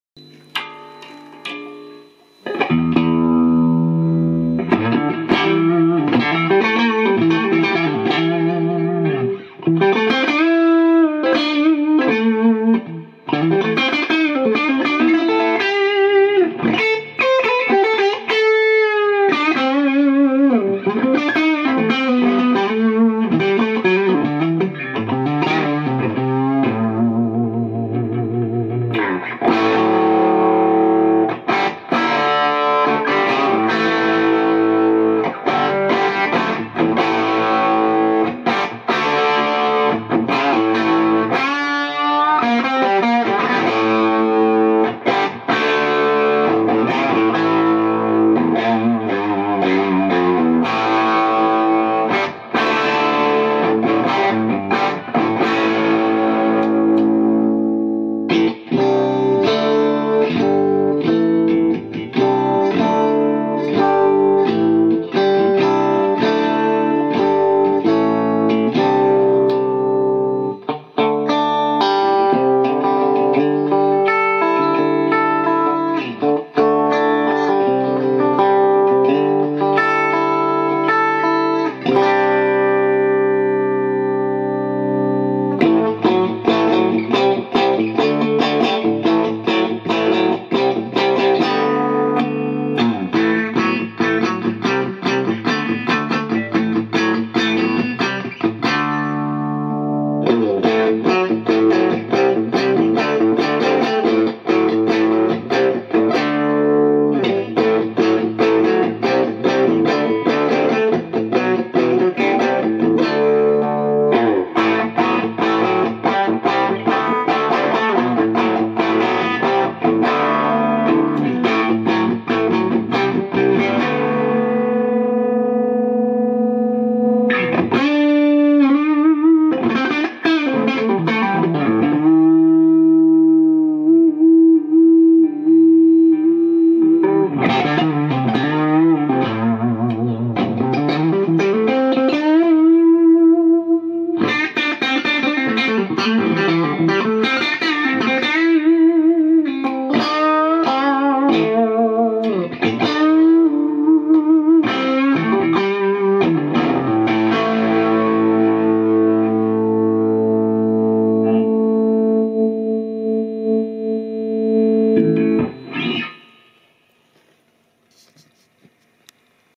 Firebird FirebEard
White FirebEard... on a laney VC30, drive channel.
3 Pickups and a smart wiring to get usable combinations
upper switch : standard 3 sounds..
lower switch up : middle pickup in phase
lower switch down middle pickup out of phase with pot blending